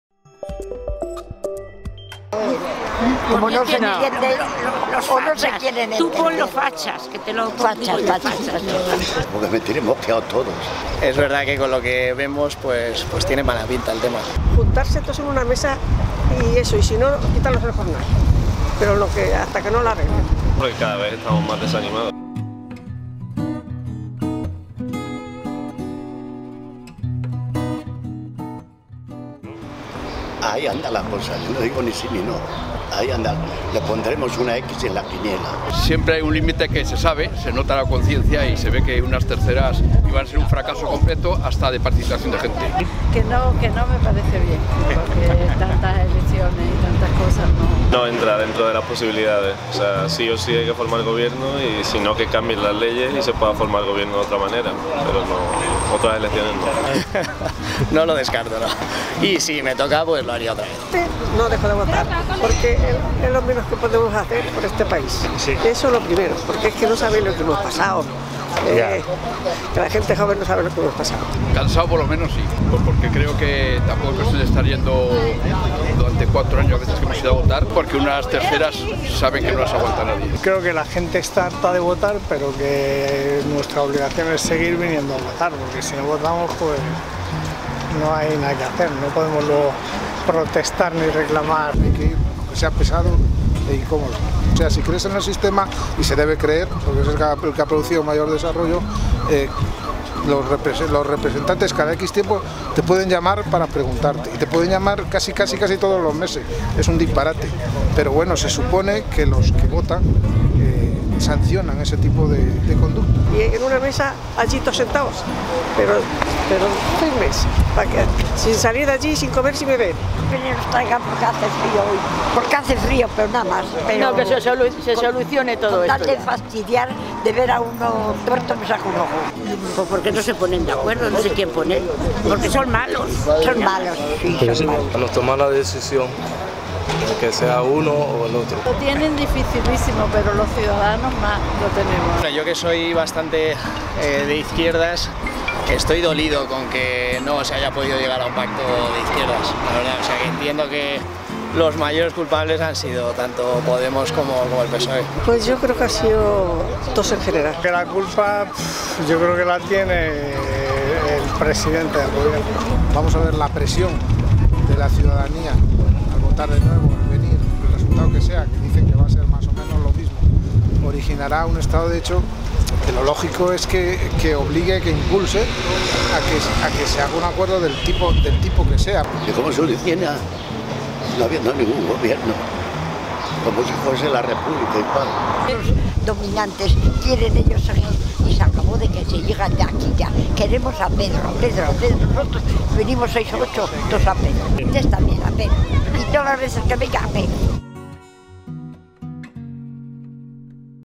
¿Y qué pasa si hay nuevas elecciones?
Hemos salido en este 10-N a la calle para preguntar a la gente sobre una posible repetición electoral.